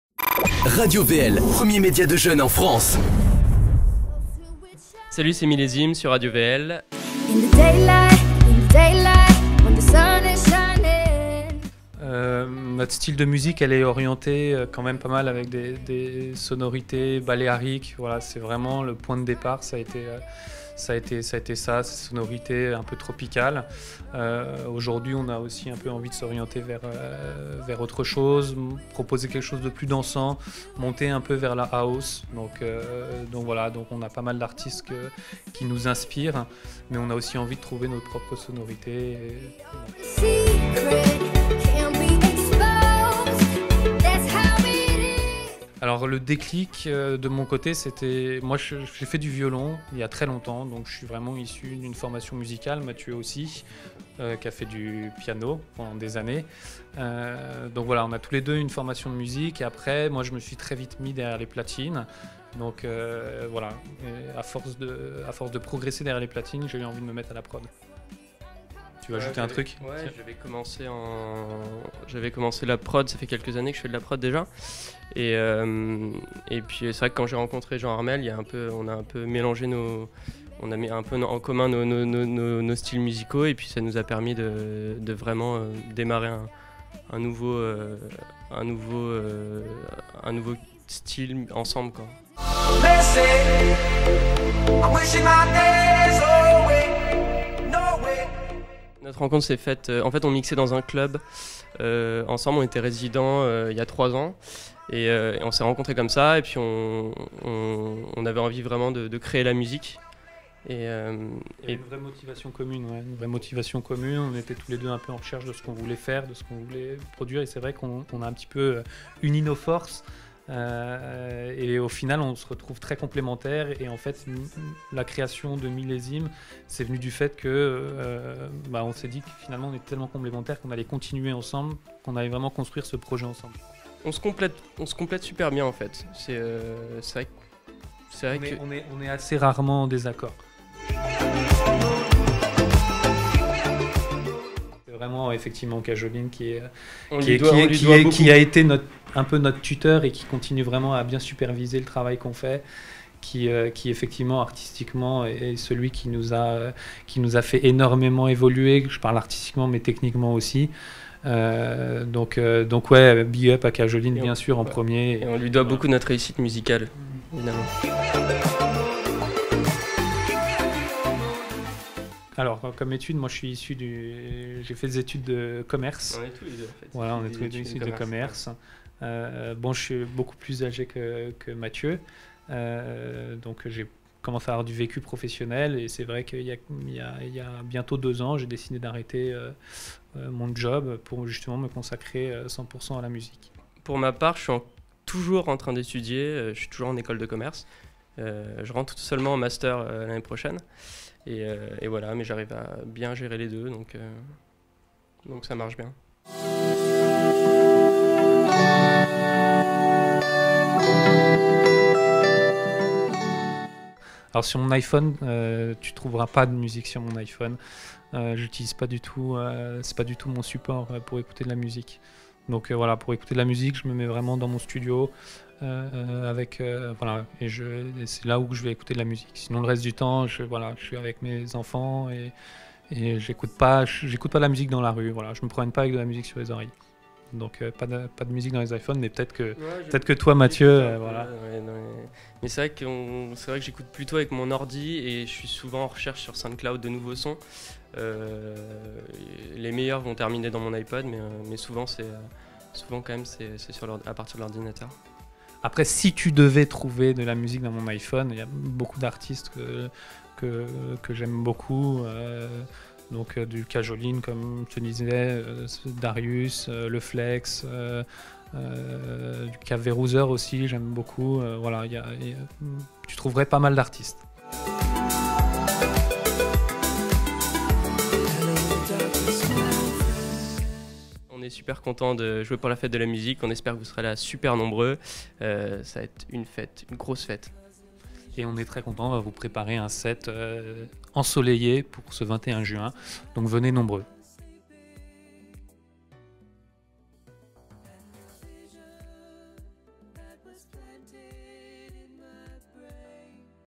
Millesim : "Nous sommes complémentaires"
Ils seront  l'affiche de la fête de la musique 2015 by Radio VL & Get the sound. Véritable amoureux de la musique et passionnés le duo évolue et surf sur la vague deep. Avec des millions d'écoutes sur les différentes plateformes internet, Millesim est le grand cru de la fête de la musique 2015.